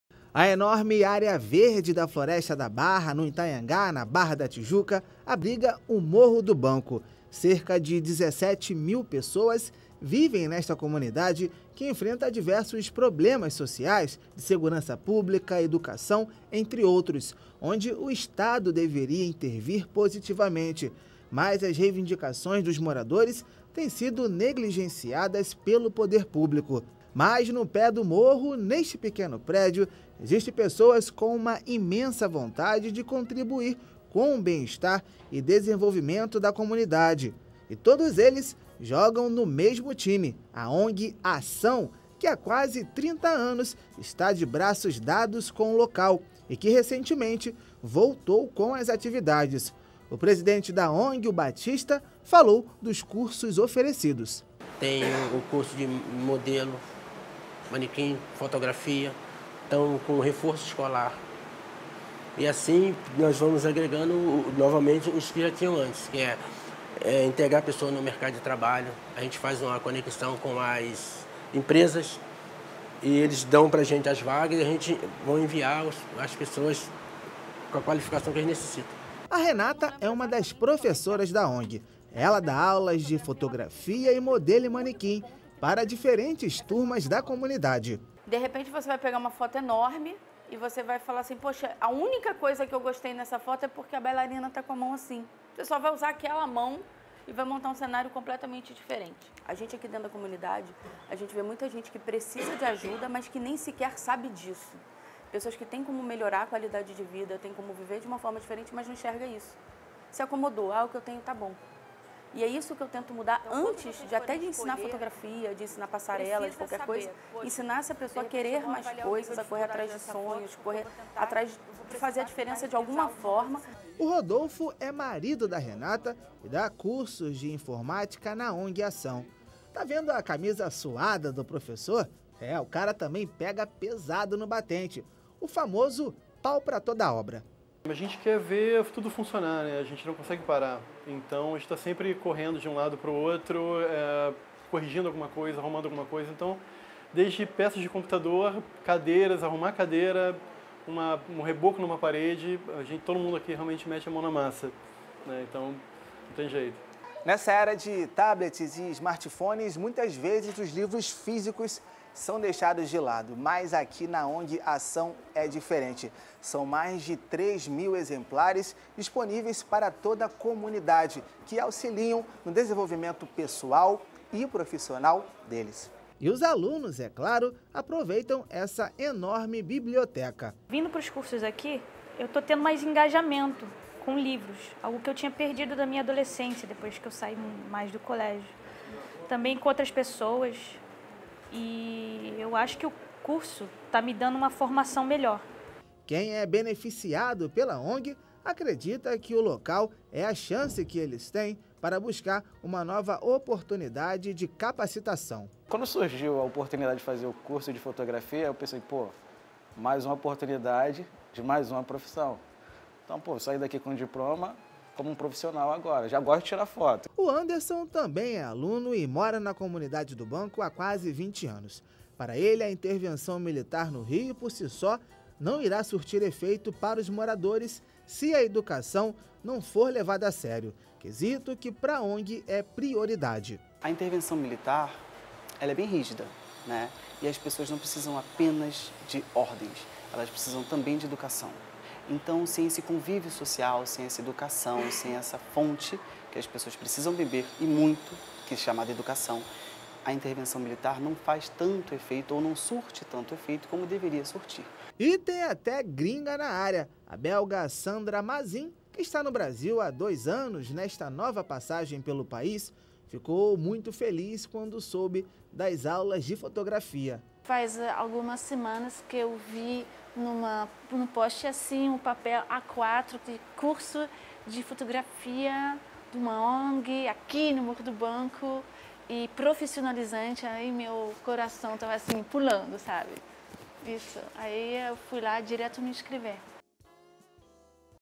ONG “Ação” ajuda moradores do Morro do Banco a ter uma boa qualidade de vida
ONG localizada na Zona Oeste do Rio, ajuda moradores de comunidades a melhorar sua qualidade de vida através de cursos de fotografia, manequim, informática e reforços escolares.